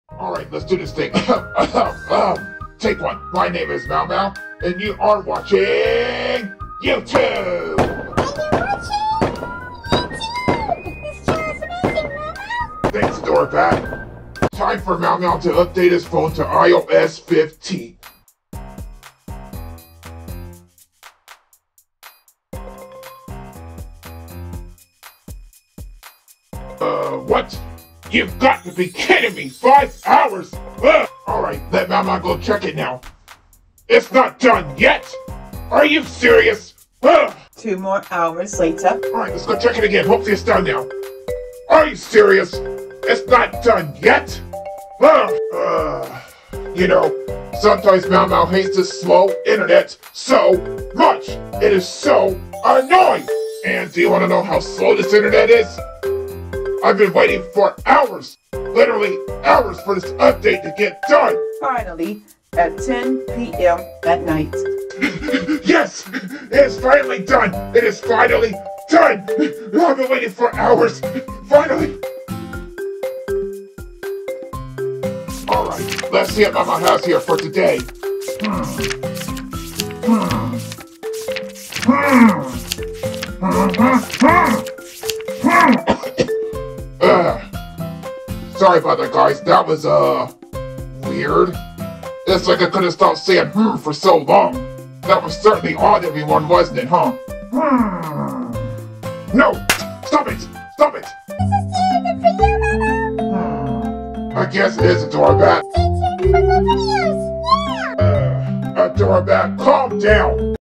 Mao Mao: When you update your phone to iOS 15 with slow internet 😂
Mao Mao’s slow internet annoys him as he updates his iPhone 12 to iOS 15